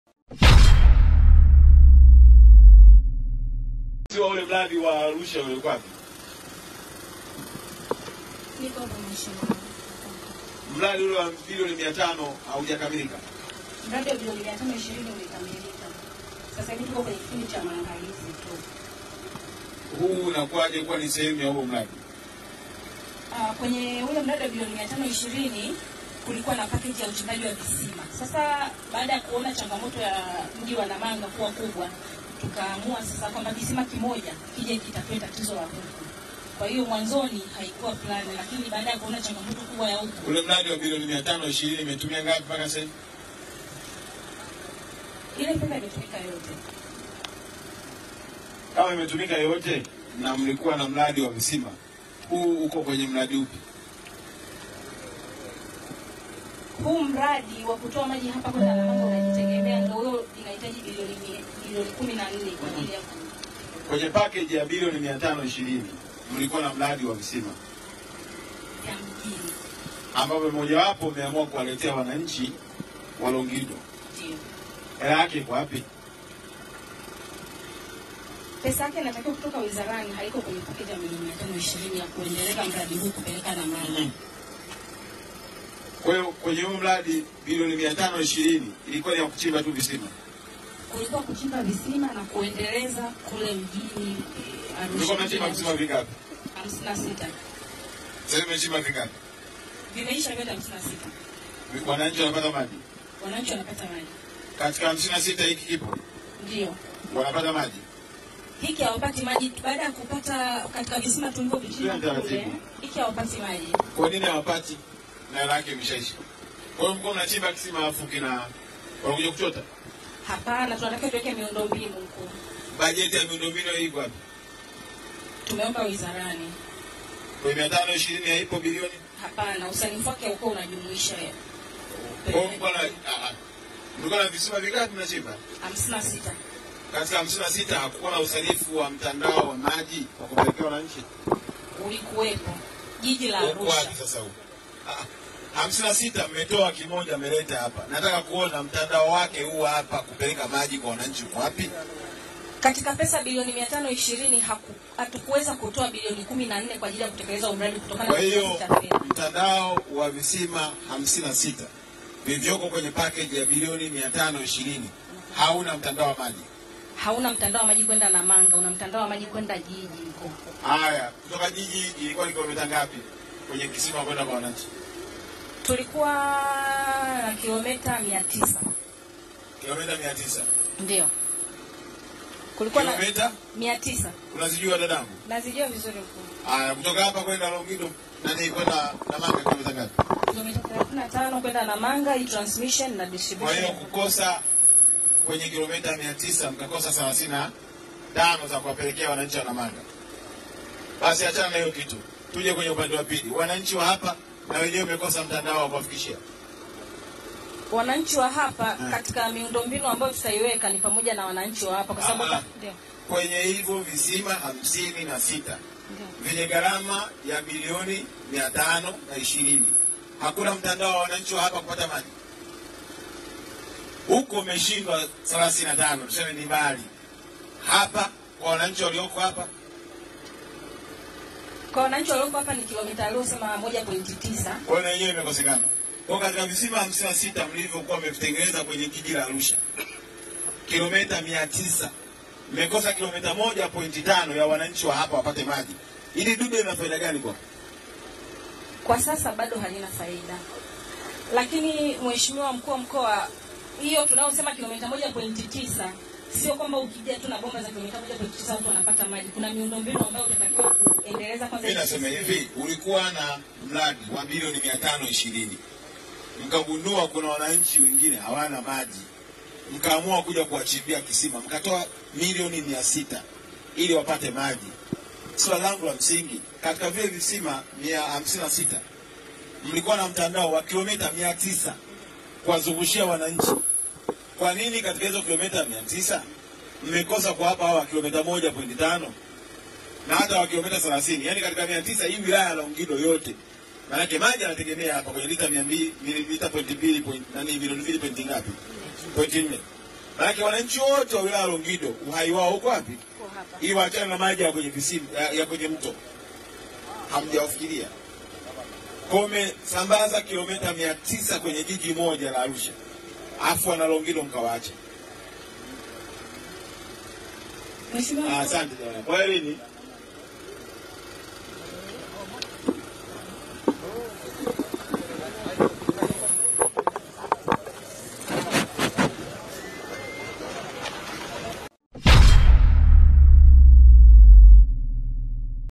MAKONDA AKASIRIKA BOSI wa MRADI wa MAJI MATATANI MASWALI MAGUMU TSH MILIONI 600 LONGIDO
Karibu kujiunga na Channel yetu ya WhatsApp kwa Habari, Matukio, Burudani

KWA HABARI ZA KITAIFA,KIMATAIFA ELIMU,MAKALA NA BURUDANI KWA UHAKIKA MASAA 24,TUFUATILIE OFFTRACK TV.